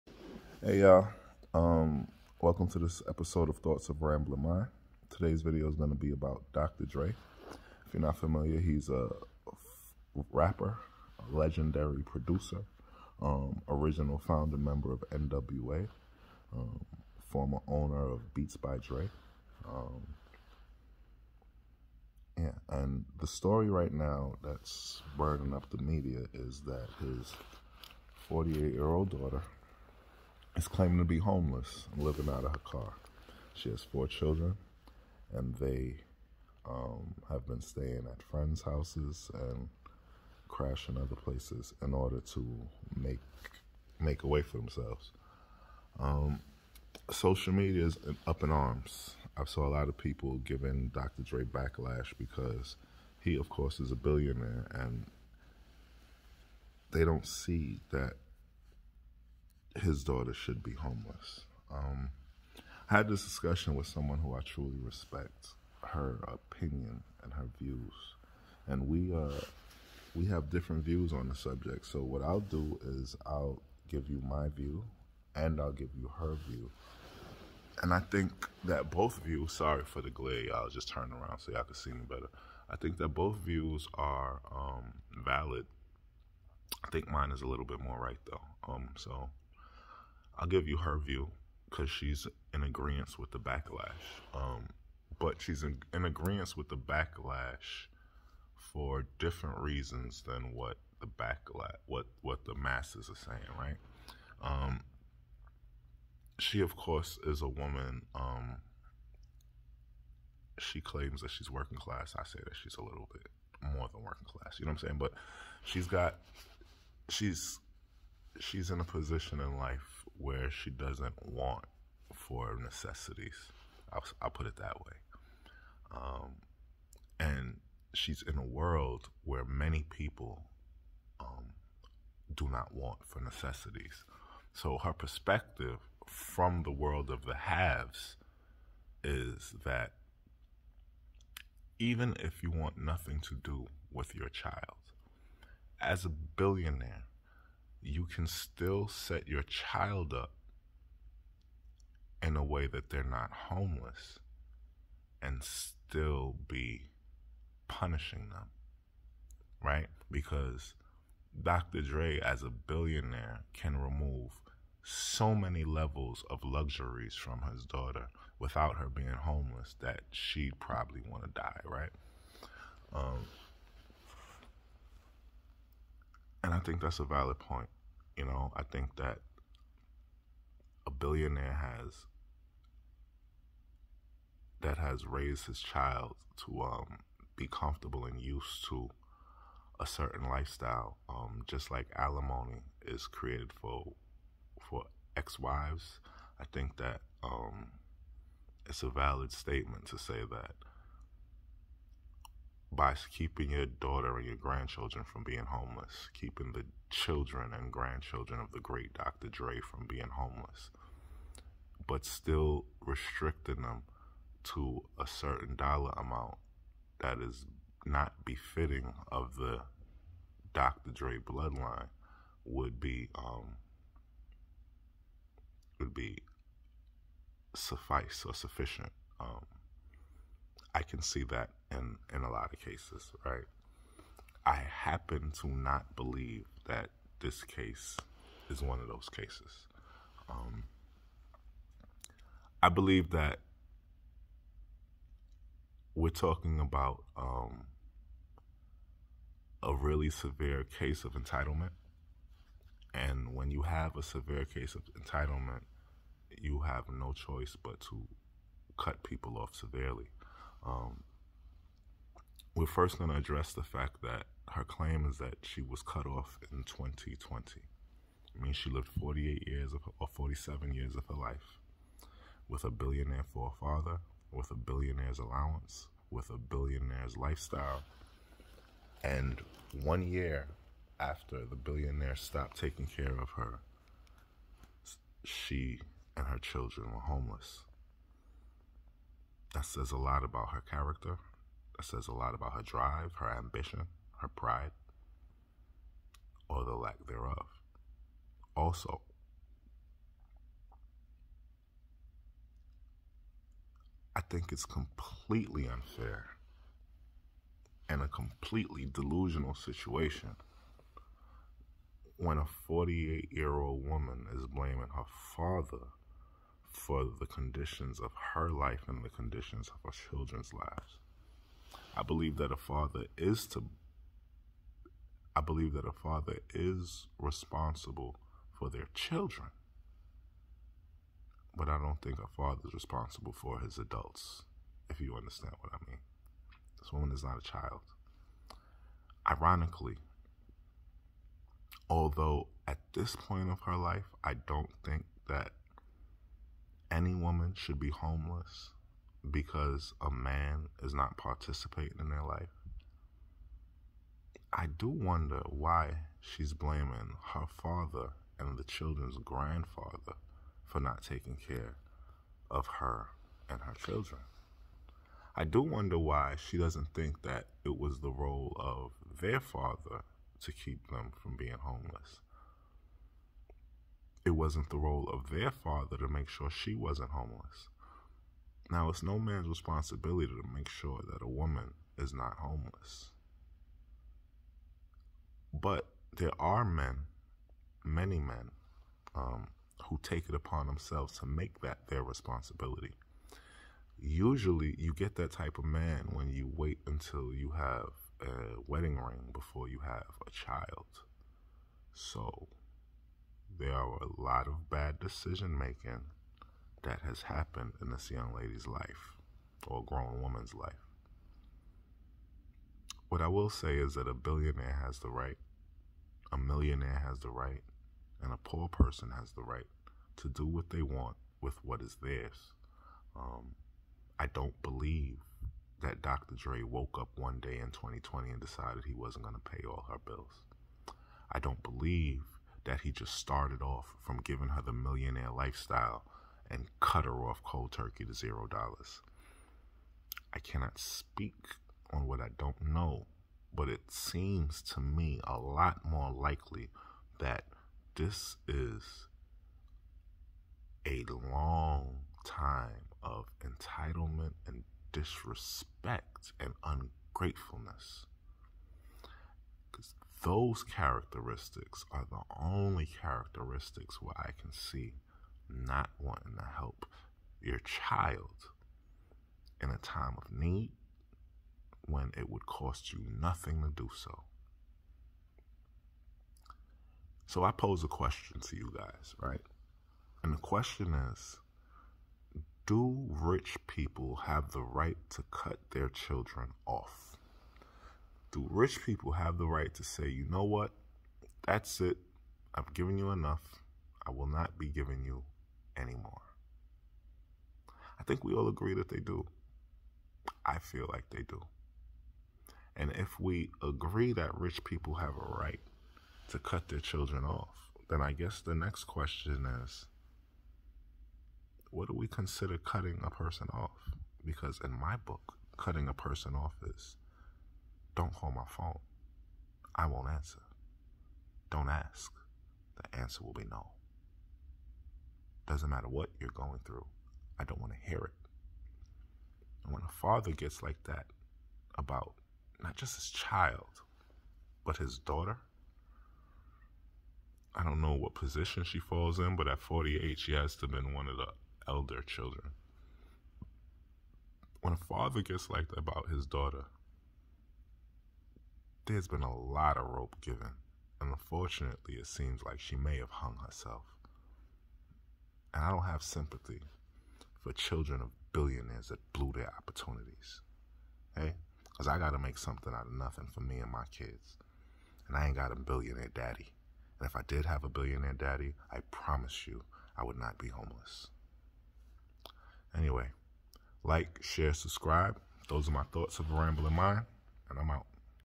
Thoughts of a Rambling Mind: "Dr. Dre's Daughter is Homeless??!!"
Rapper Dr Dre is facing backlash overv his daughters claims